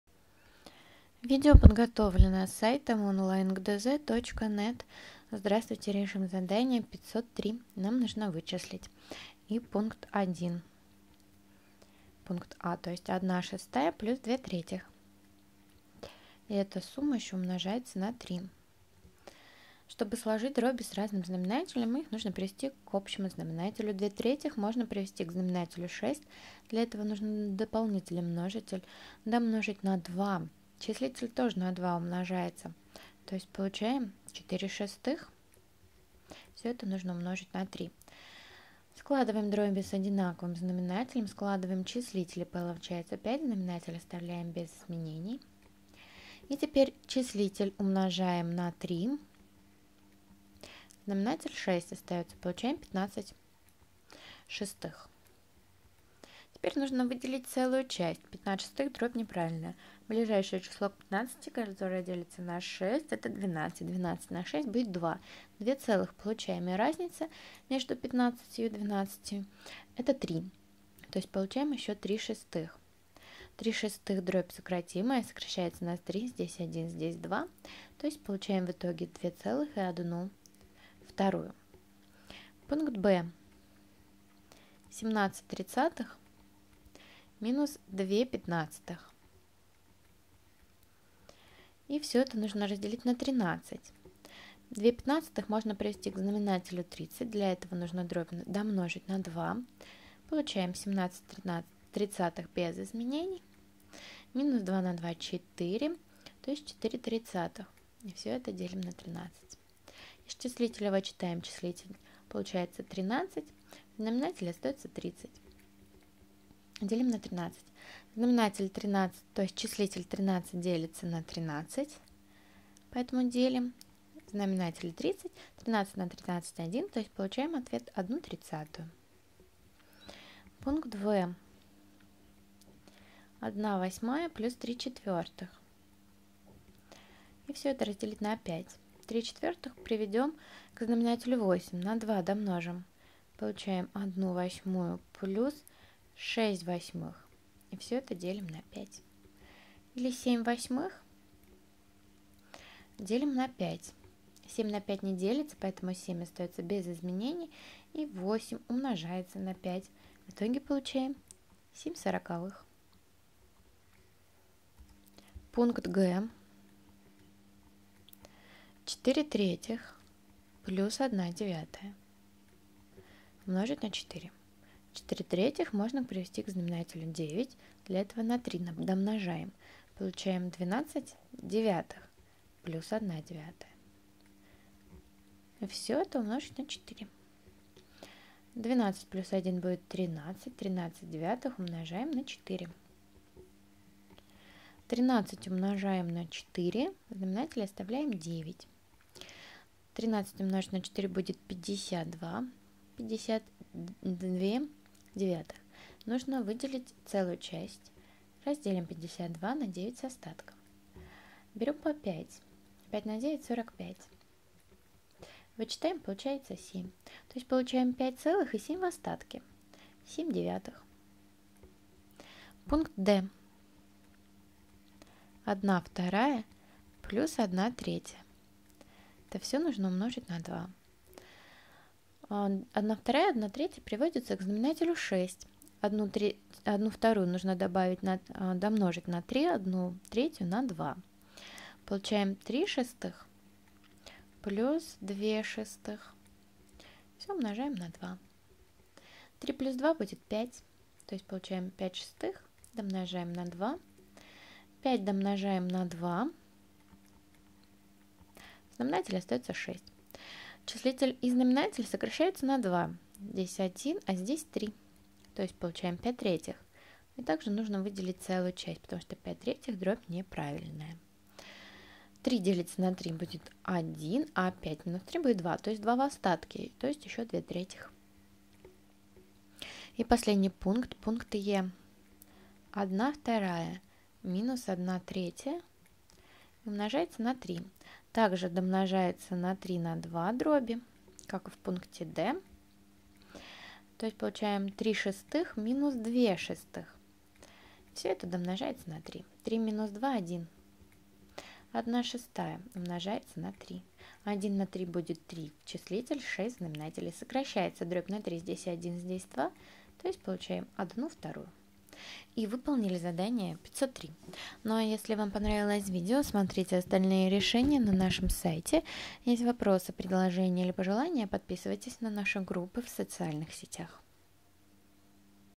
Задание № 503 - Математика 5 класс (Зубарева, Мордкович)
В задании 503 вам даны шесть выражений, значения которых нужно вычислить. А для этого нужно вспомнить правила сложения и вычитания дробей с одинаковыми и разными знаменателями, и те, правила, которые вы изучили в этом параграфе – деление и умножение обыкновенной дроби на натуральное число. Чтобы сделать это задание без ошибок, воспользуйтесь нашим видео с решением.